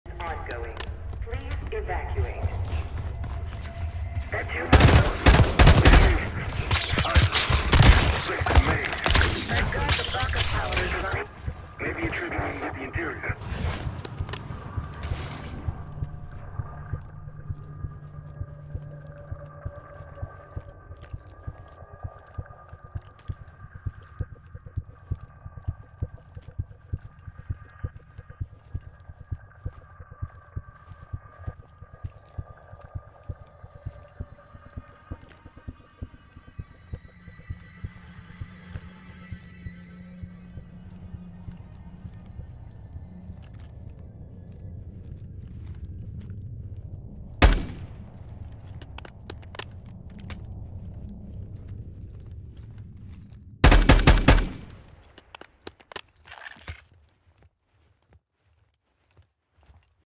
that feeling when you found a glitch spot
Fell into the void, yeah my dumbass think this is cool